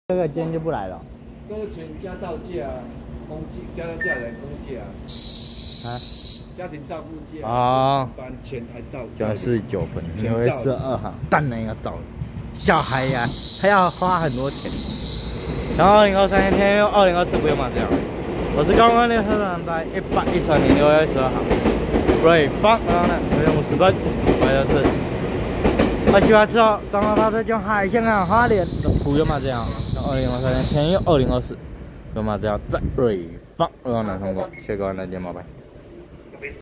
278次彰化發車經由海線開往花蓮的TEMU2023+TEMU2024普悠瑪自強號
112/6/12瑞芳站9:50分拍攝278次彰化發車經由海線開往花蓮的TEMU2023+TEMU2024普悠瑪自強號